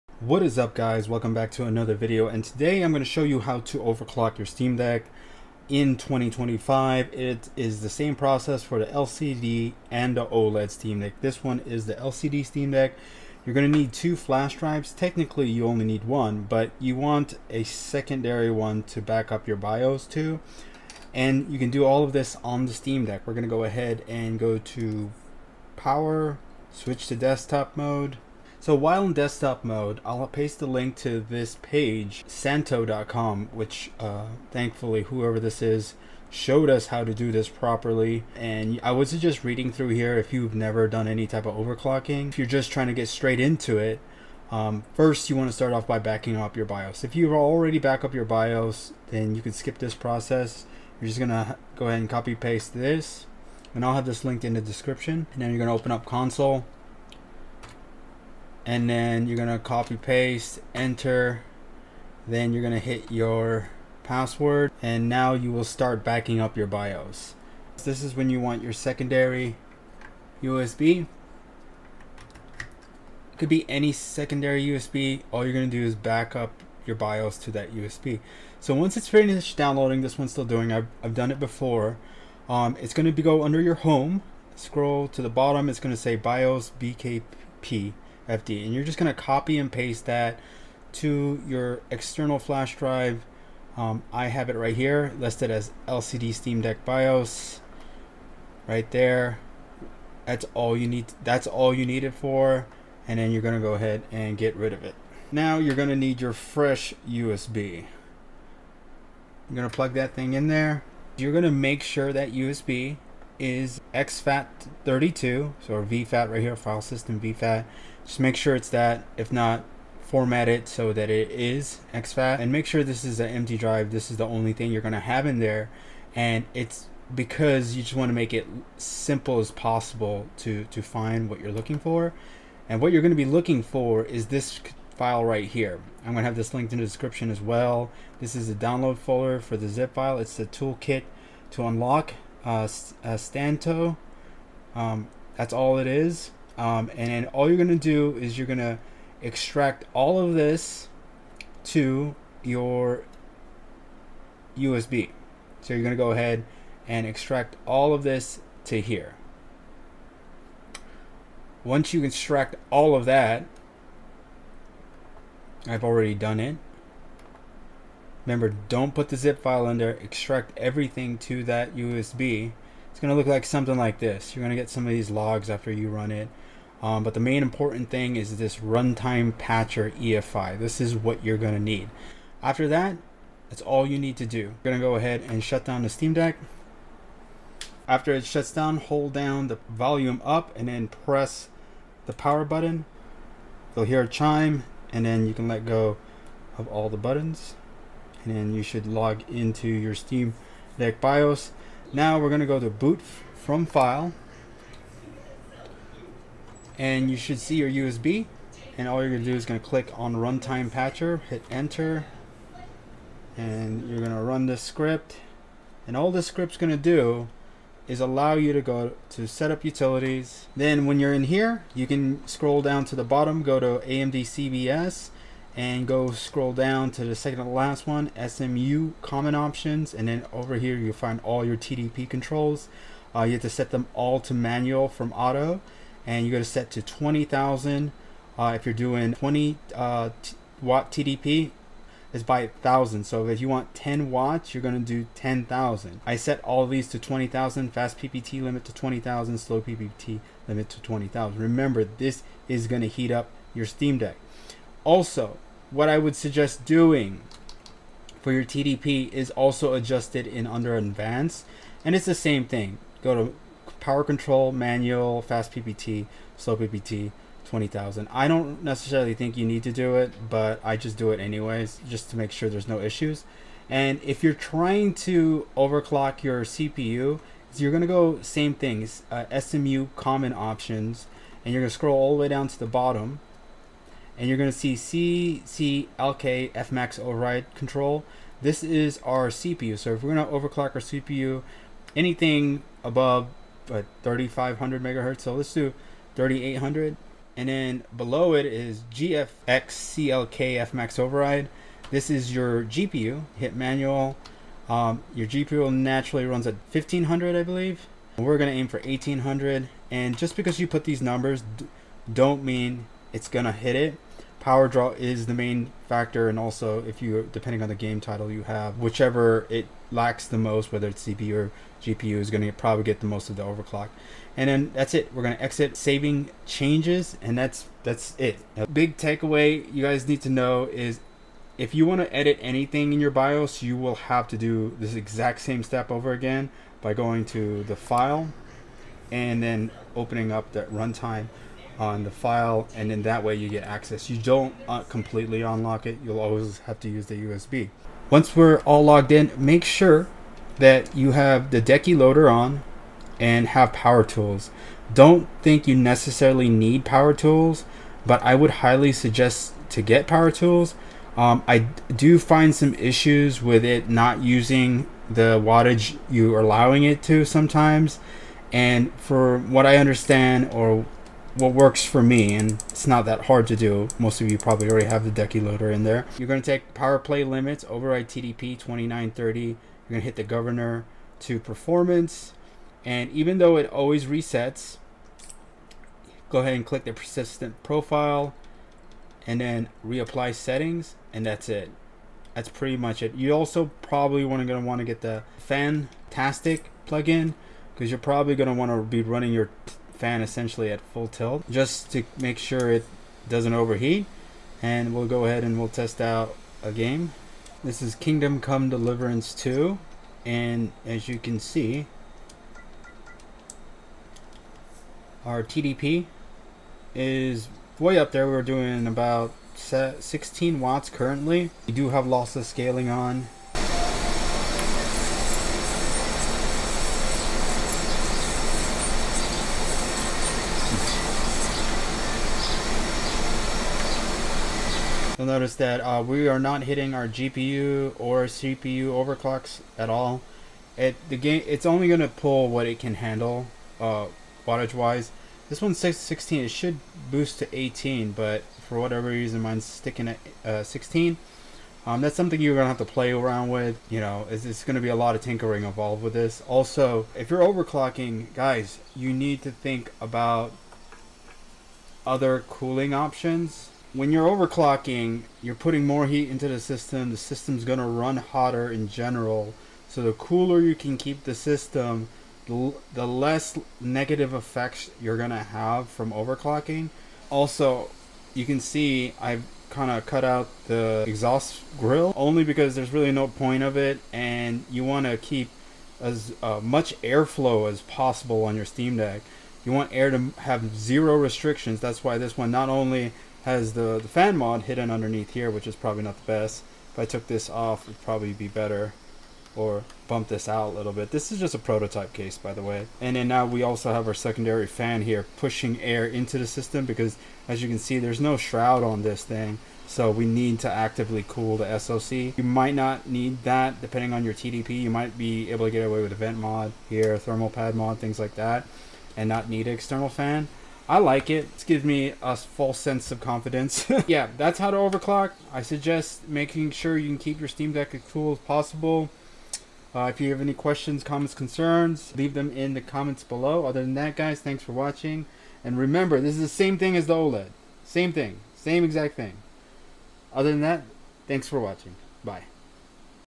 Steam Deck Overclocking OLED / LCD
How to Overclock your Steam deck

This is the Guide I used to overclock, if you are new to this I suggest you read it first.

These steps work for both LCD and OLED Steam Deck

Warning overclocking will degrade the lifespan of your Stem Deck and void your warranty

Step 1 - Back up Bios

  While on desktop mode, open Konsol and type in

  sudo /usr/share/jupiter_bios_updater/h2offt /home/deck/biosbkp.fd -O

  This will download into your home folder, copy it to a separate drive

  Extract to a UBS that is formatted to FAT32

Step 3 - Turn off the steam deck

   Then log into the bios buy pressing the Volume button and power at the same time

  Then on the bottom left choose BOOT FROM FILE

  Then find your USB drive with above file, open file and open RUNTIME PATCHER